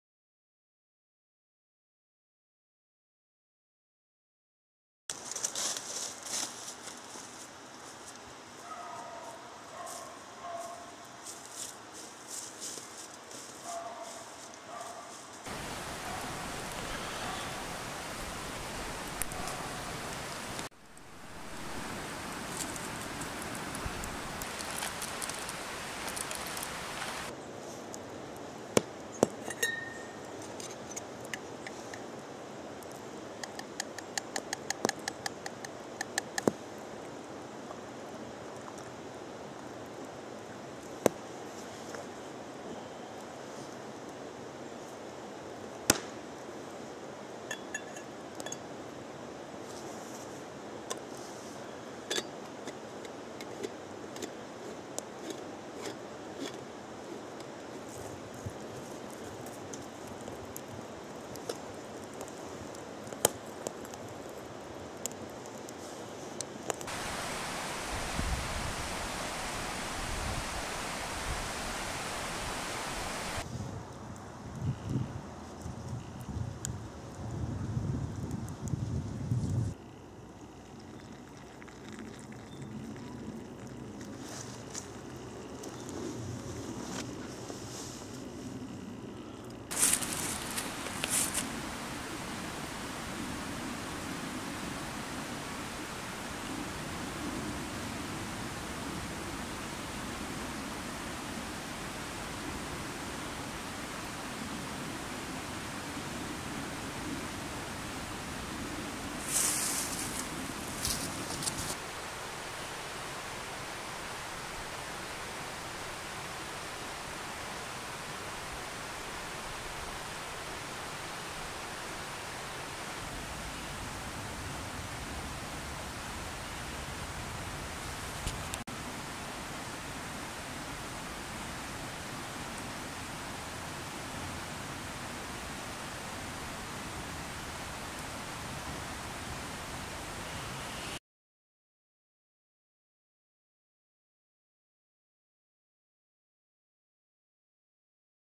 Espresso in the woods
こんな旨いエスプレッソは他には無い！と思いました。
この日のためにスペシャルブレンド＆ローストしていただいたNapoletano。

お近くへお越しの際はぜひ立ち寄ってあげてくださいね。